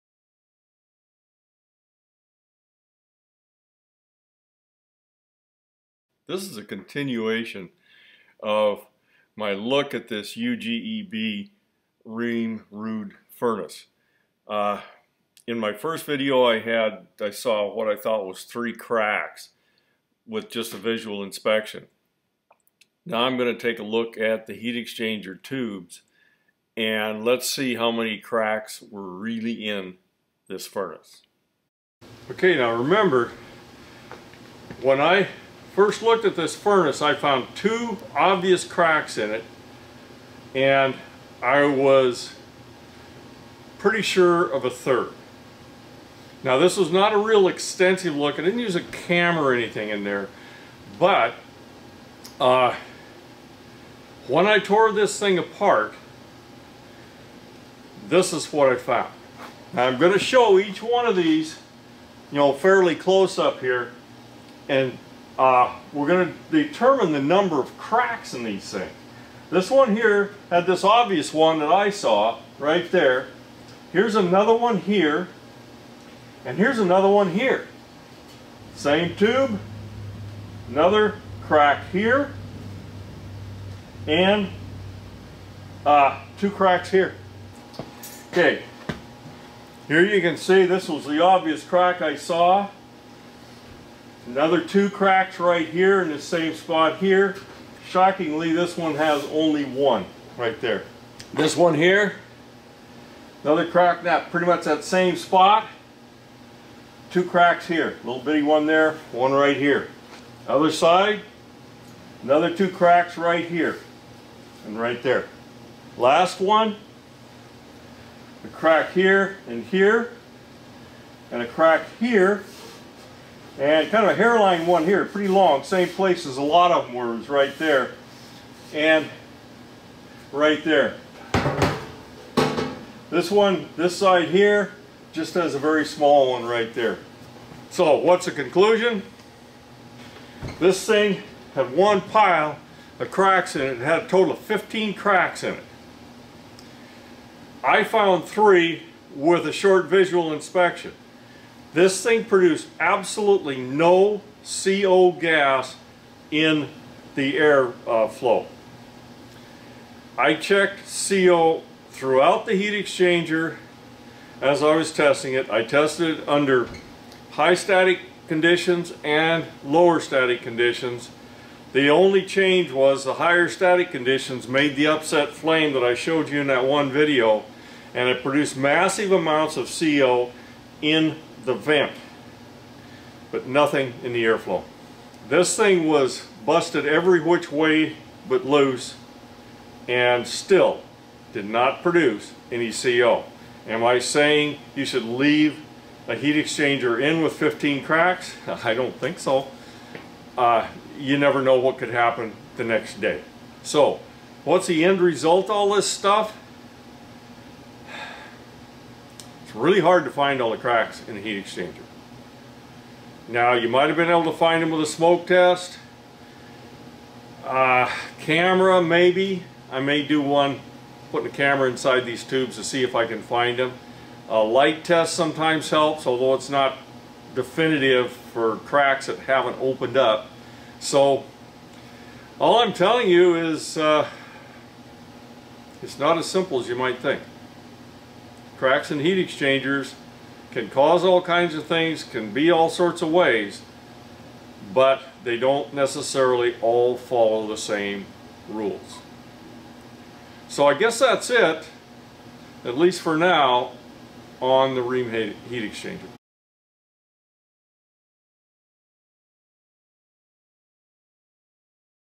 The cracked heat exchanger
This one takes apart the Ruud/Rheem heat exchanger to inspect the cracks.  This video is part of the heating and cooling series of training videos made to accompany my websites to pass on what I have learned in many years of service and repair. If you have suggestions or comments they are welcome.